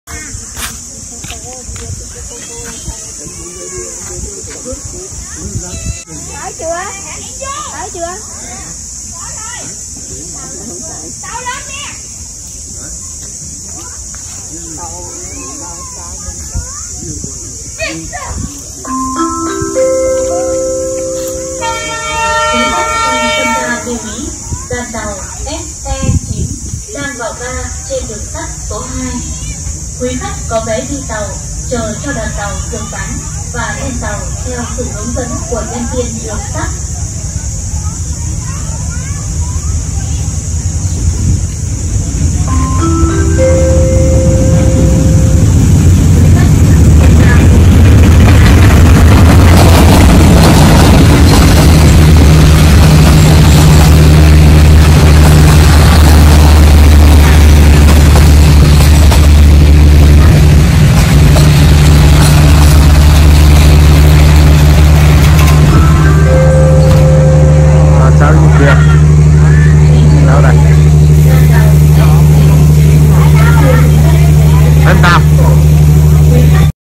Tàu SE9 vào ga Đức Phổ
Mình đang chuẩn bị lên tàu tại ga Đức Phổ để vào Sài Gòn. Các bạn có biết Ga Đức Phổ không?

Ga Đức Phổ là một nhà ga xe lửa tại thị xã Đức Phổ, tỉnh Quảng Ngãi. Nhà ga là một điểm của đường sắt Bắc Nam và nối với ga Thạch Trụ với ga Thủy Trạch.

Ga Đức Phổ phục vụ cho hơn 140.000 dân rải khắp trên toàn 182 phường xã của Thị Xã Đức Phổ. Ga Đức Phổ cách ga Hà Nội 968km, nhà ga mỗi ngày đón 2 chuyến tàu chạy hướng Đức Phổ - Hà Nội và 2 chuyến ngược lại với điểm cuối là ga Sài Gòn.